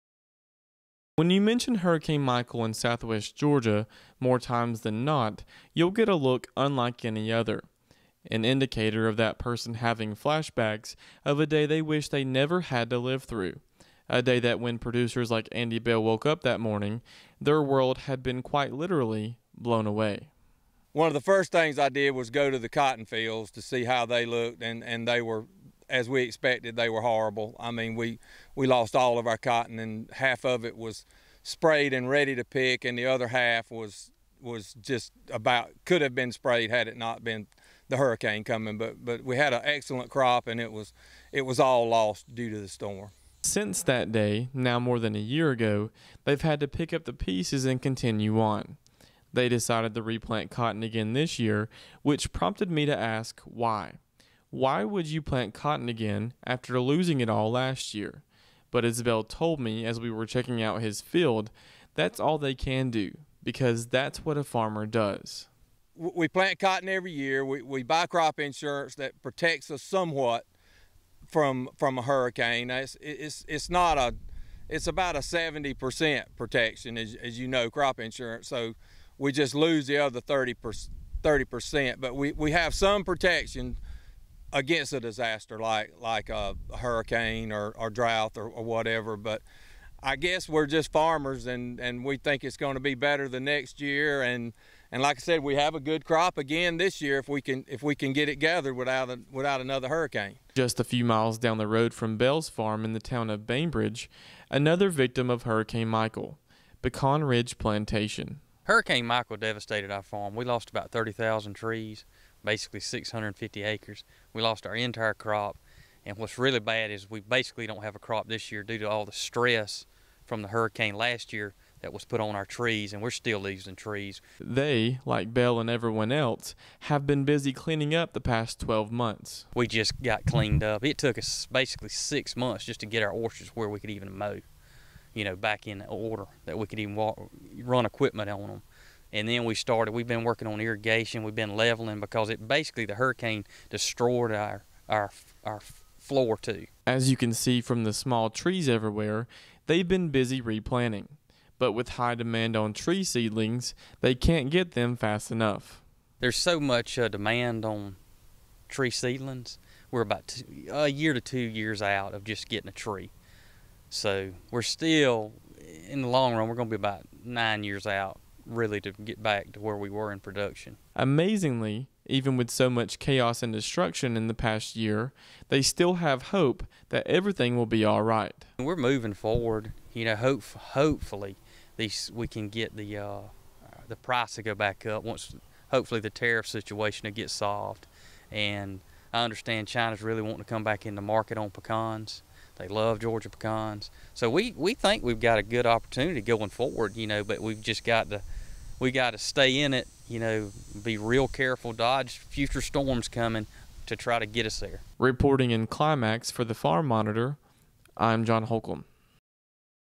Farmers Still Reeling One Year After Hurricane Michael
ONE COULD CERTAINLY SAY THAT HURRICANE MICHAEL LEFT A LASTING MARK ON GEORGIA'S AG INDUSTRY. THE DOLLAR FIGURE, IN THE BILLIONS! PECAN TREE’S RIPPED FROM THEIR ROOTS. PEANUTS DESTROYED BY MICHAEL'S POUNDING RAINS. AND COTTON, SCATTERED FOR MILES. JOHN HOLCOMB SPENT TIME WITH PRODUCERS WHO ARE STILL PICKING UP THE PIECES ONE YEAR LATER